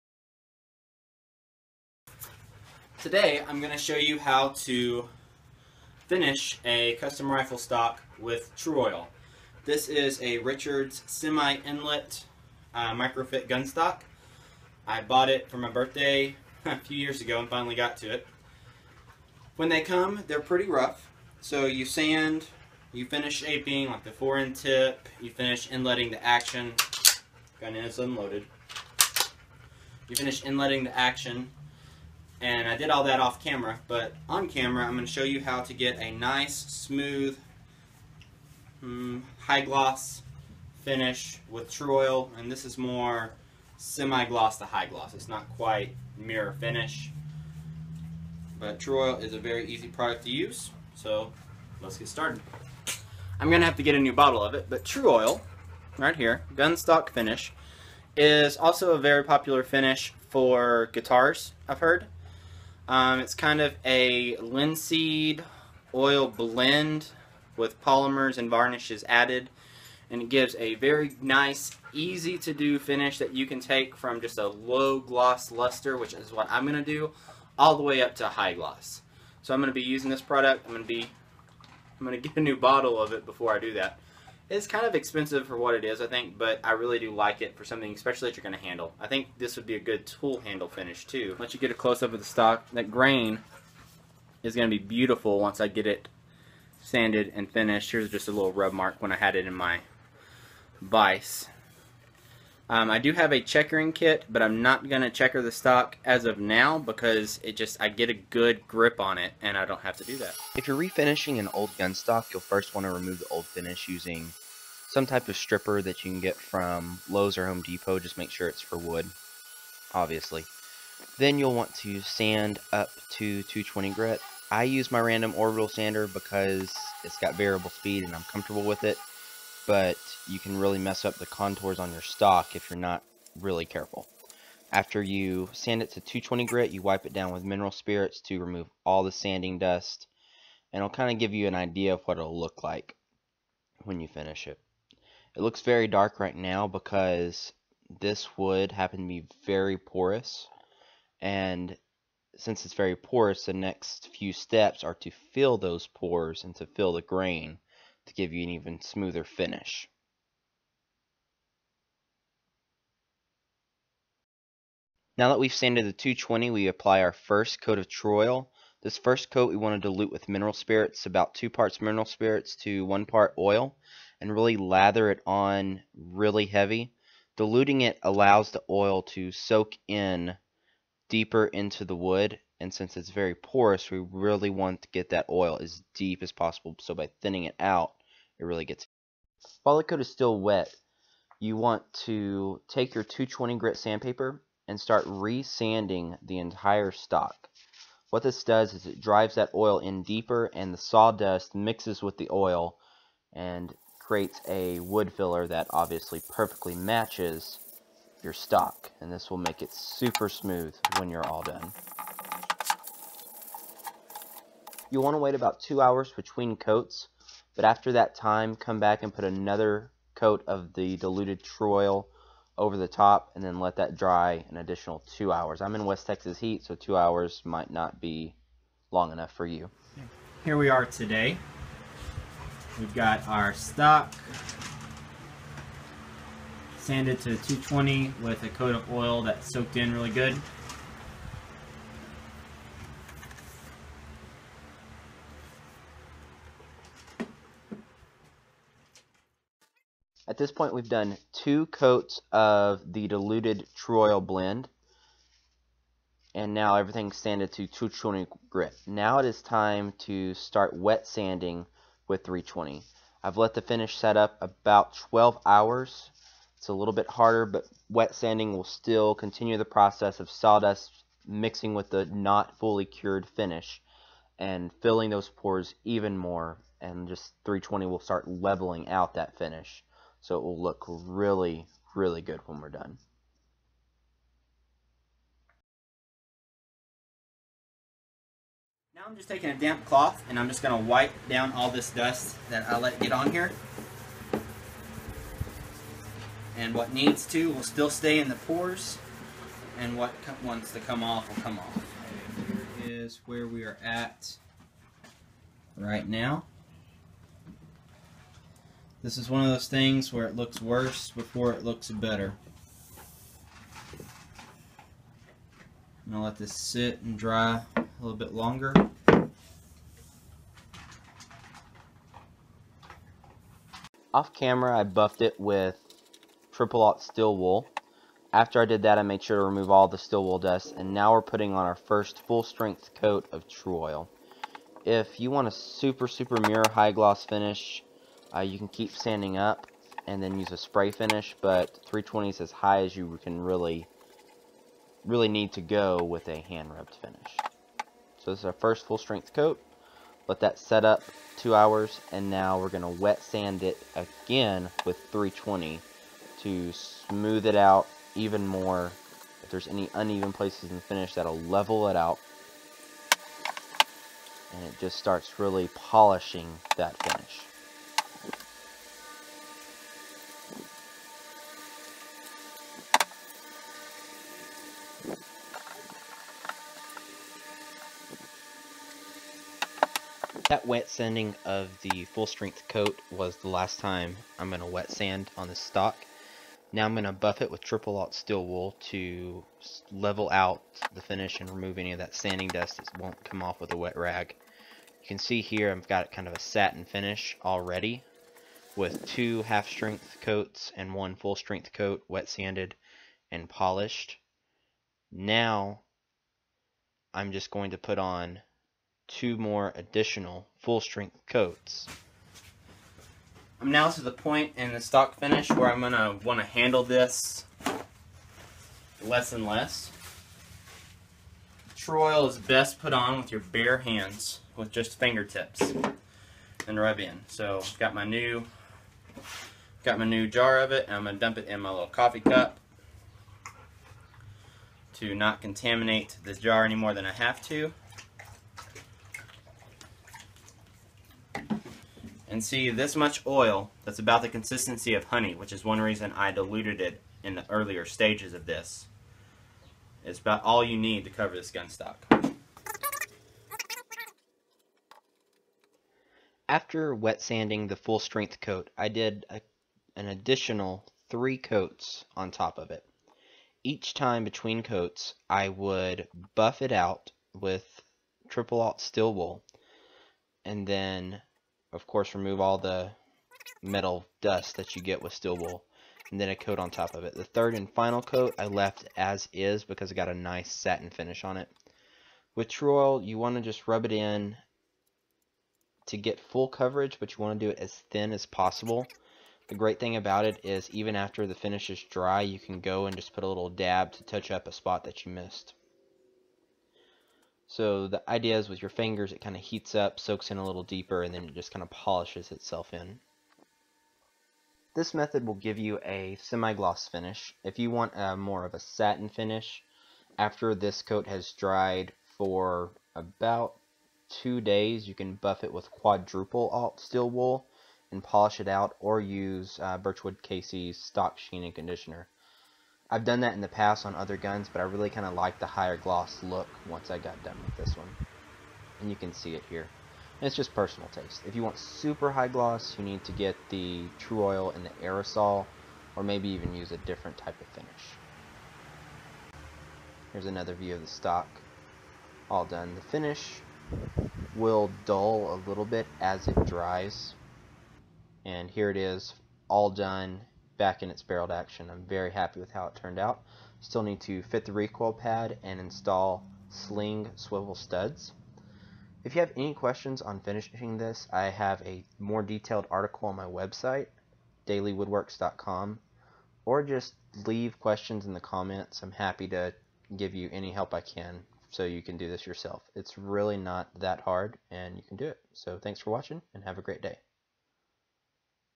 Finishing a Gun stock with Tru Oil - Complete How To (The Recreational Woodworker)
I show you how to finish or refinish a rifle stock with Birchwood-Casey Tru-oil Gun Stock Finish.  Tru-oil is also a popular guitar finish.  Using a few different finishing techniques you can have a satin to a high-gloss finish.  It's easy to do and you don't need a lot of experience to do it right.

Do you want to connect with other woodworkers?

Do you want to learn woodworking? Get plans, tool reviews, tutorials, and free content